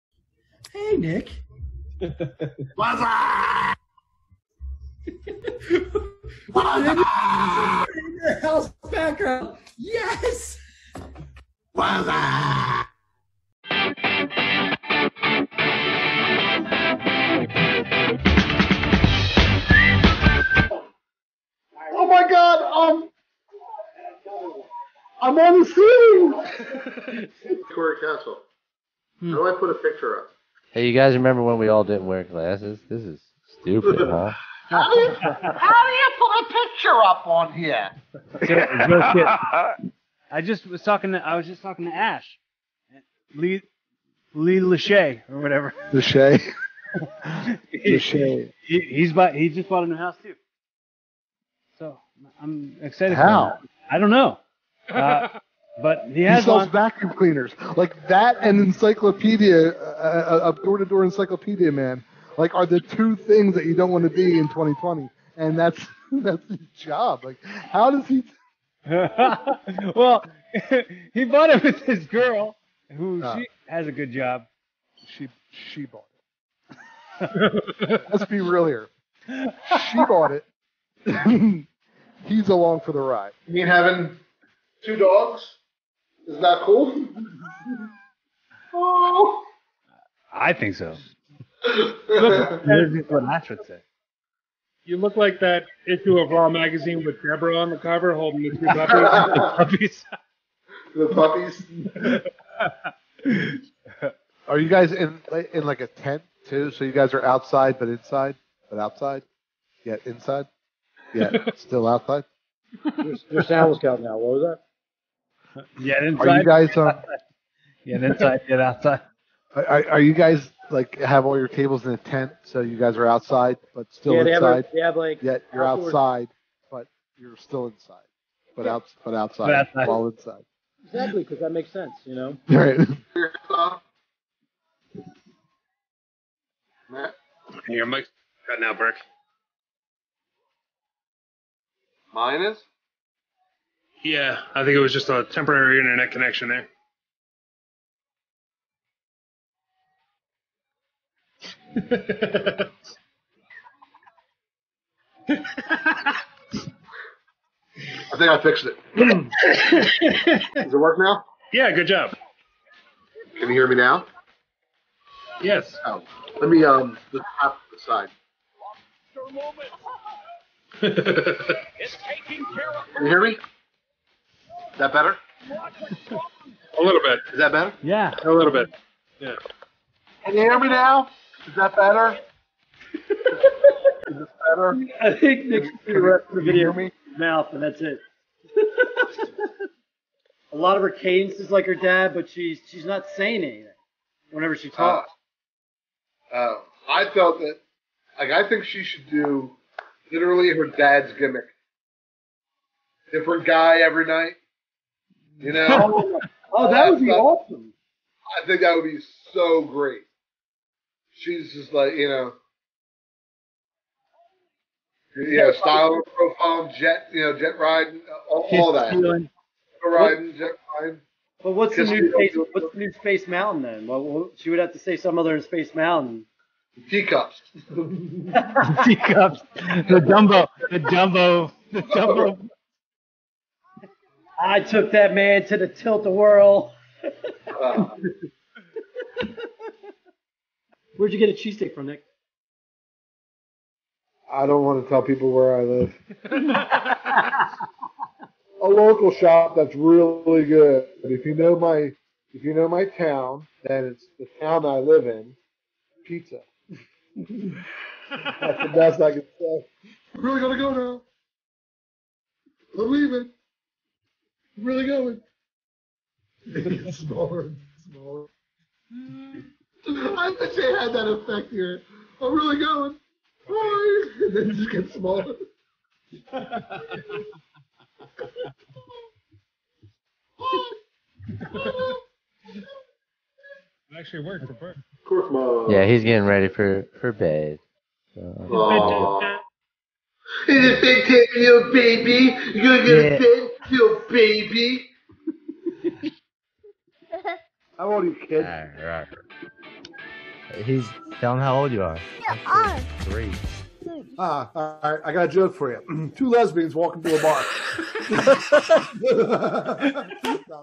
RASSLE ROCK | Best Of Nick Berk
Nick Berk has been a staple in the Rassle Rock podcast from the very beginning, so much that we end every show with him singing! Here are some of the best clips from Nick Berk from the various shows and live streams that we've done!